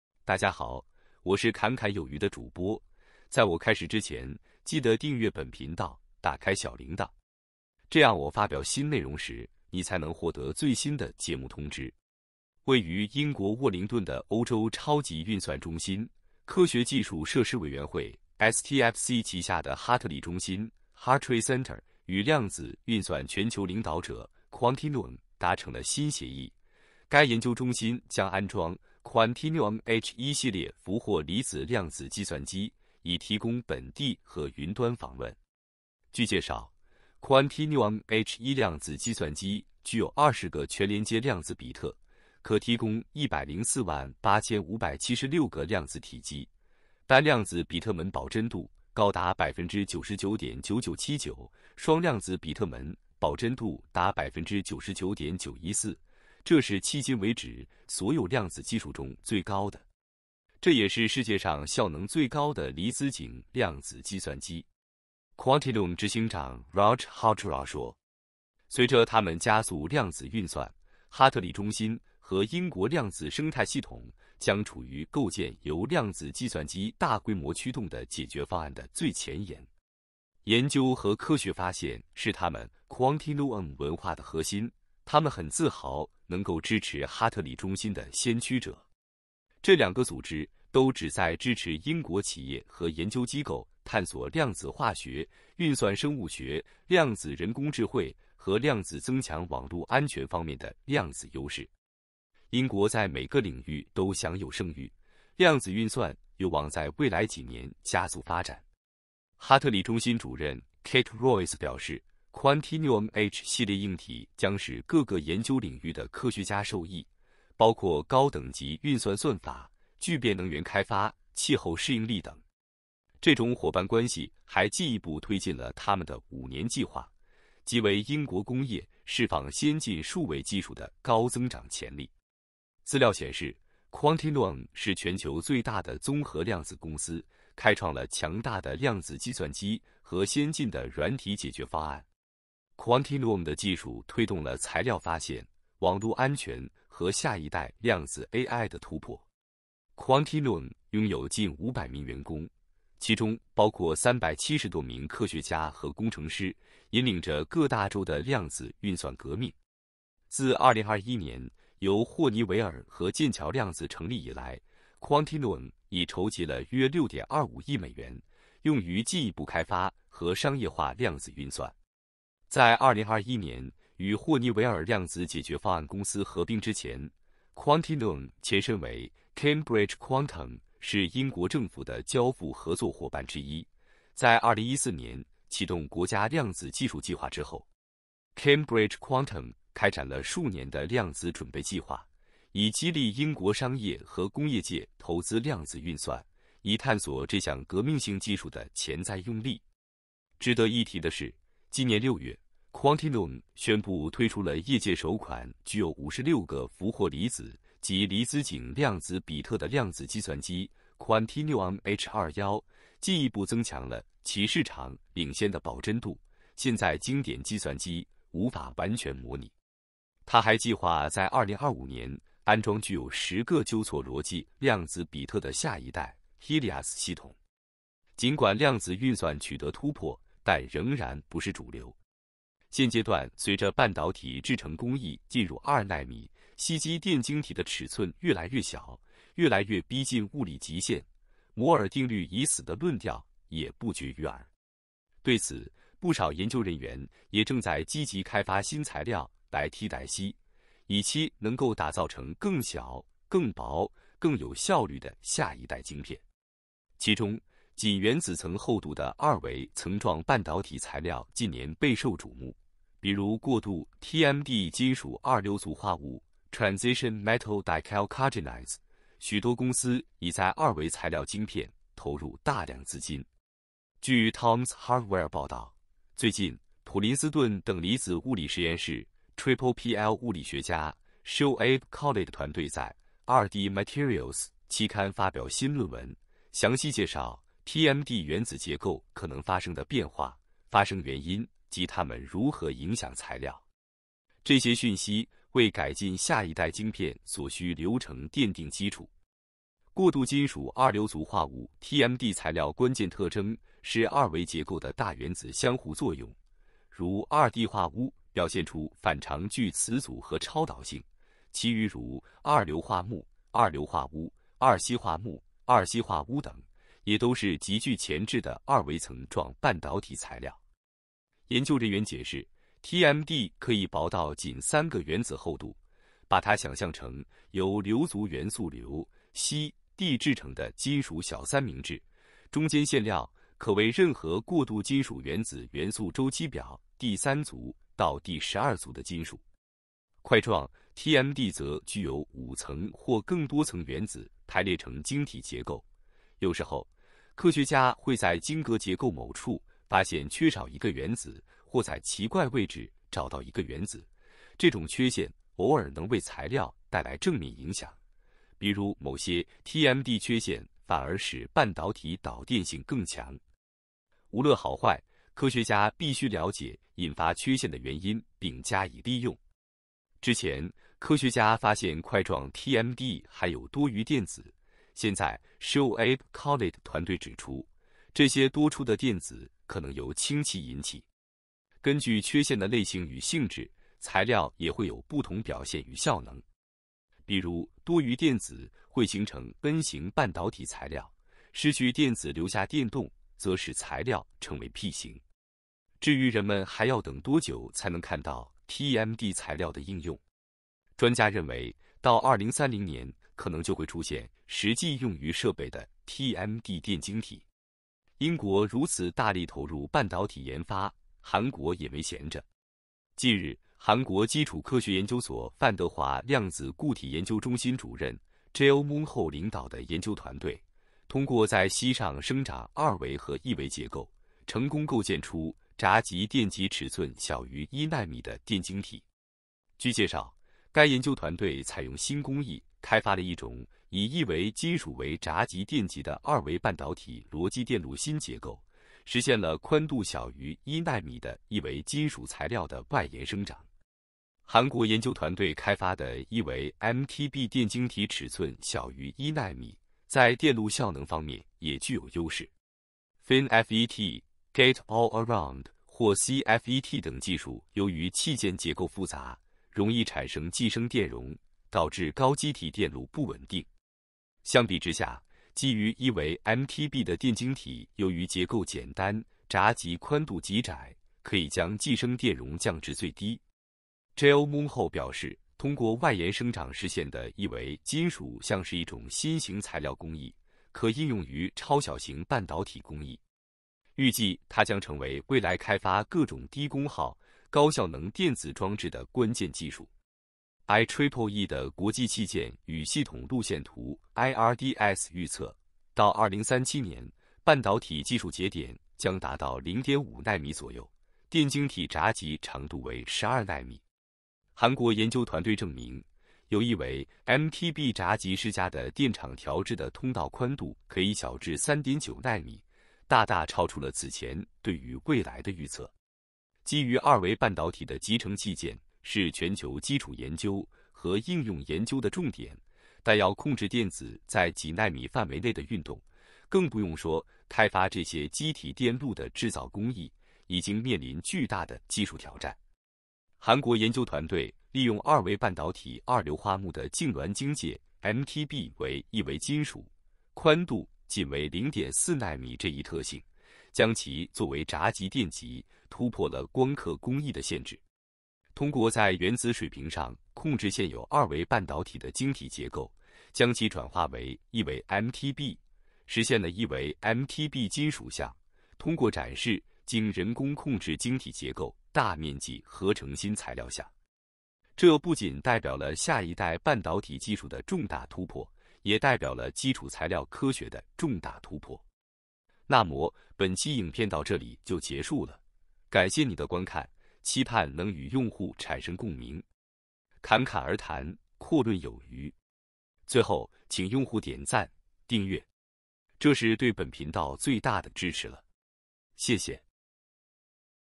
重大突破！韓國成功構建出小於1奈米的MTB電晶體，英國全新TMD材料不服，僅3個原子厚。Quantinuum H1量子計算機，入駐英國超算中心。過渡金屬二硫族化物，極具潛質的二維層狀晶片材料。
英國在每個領域都享有盛譽，量子運算有望在未來幾年加速發展。为支持英國企業和研究機構探索量子化學、運算生物學、量子人工智慧和量子增強網路安全方面的量子優勢。Quantinuum執行長Raj Hazra說。隨著他們加速量子運算，哈特里中心和英國量子生態系統將處於構建由量子計算機大規模驅動的解決方案的最前沿。據Tomshardware報道，最近，普林斯頓等離子物理實驗室PPPL物理學家Shoaib Khalid團隊在《2D Materials》期刊發表新論文，詳細介紹TMD原子結構可能發生的變化、發生原因及它們如何影響材料。近日韓國基礎科學研究所範德華量子固體研究中心主任JO Moon-Ho領導的研究團隊，通過在矽上生長二維和一維結構，成功構建出閘極電極尺寸小於1nm的電晶體。